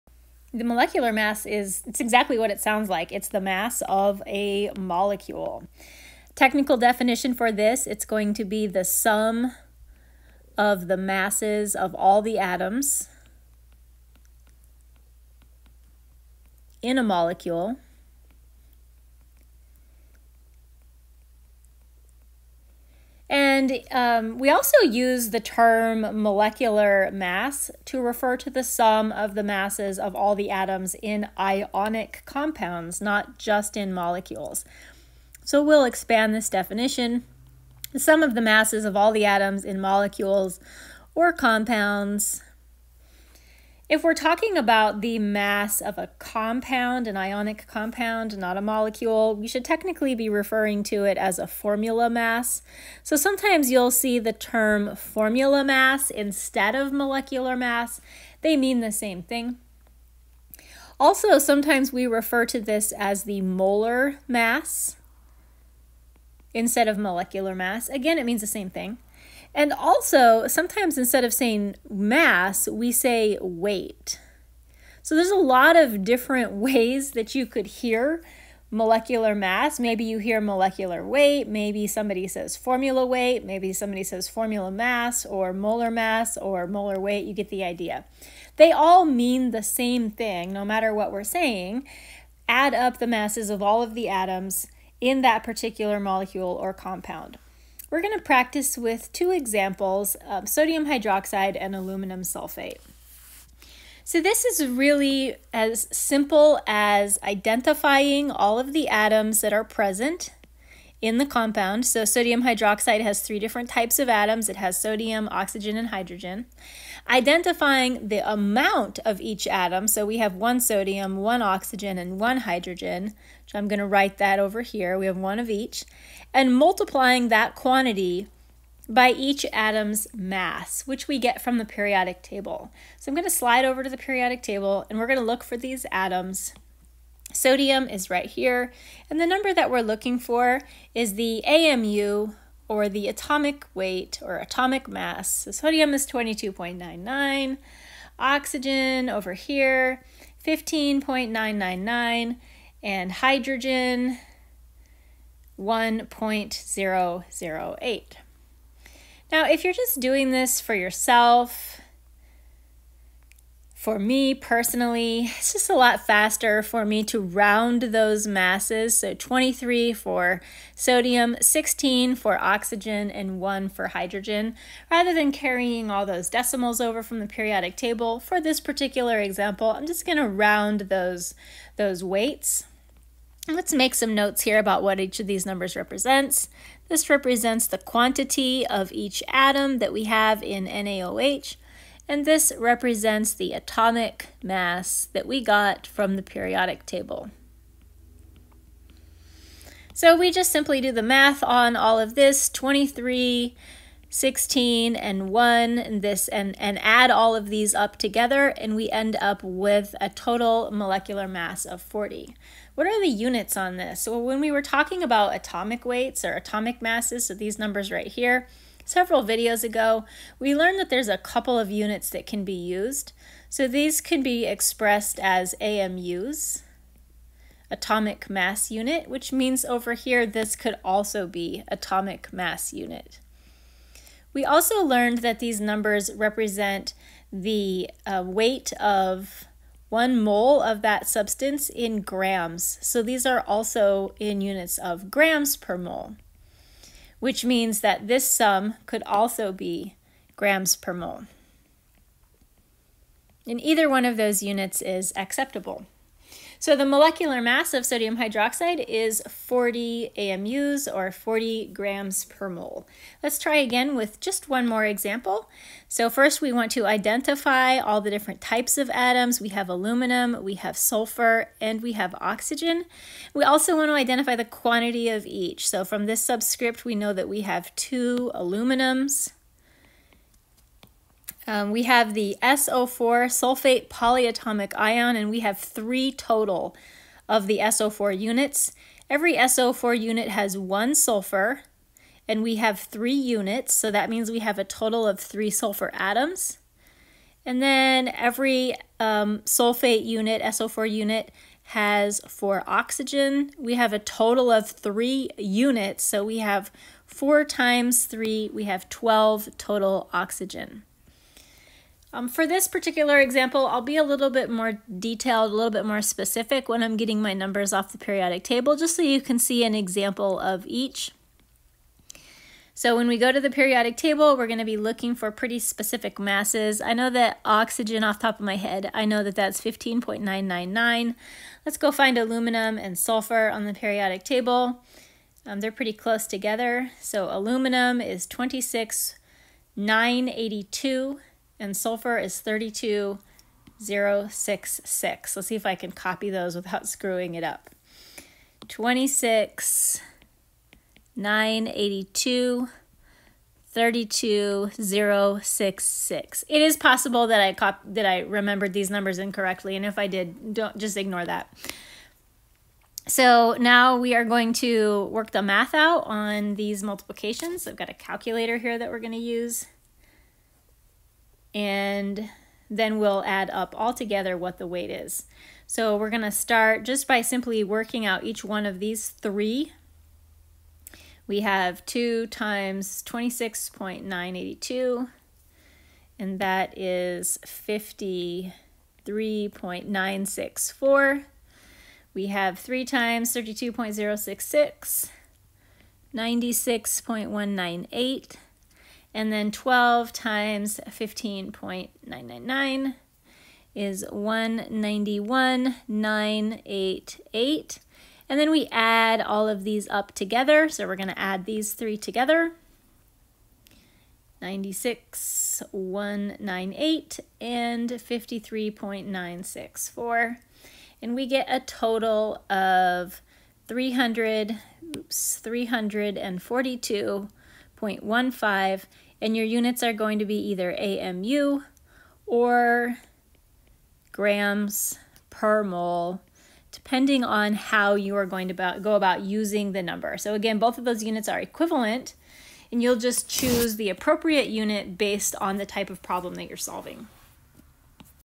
56: Introduction to molecular mass
Detailed instruction of how to calculate the molecular mass.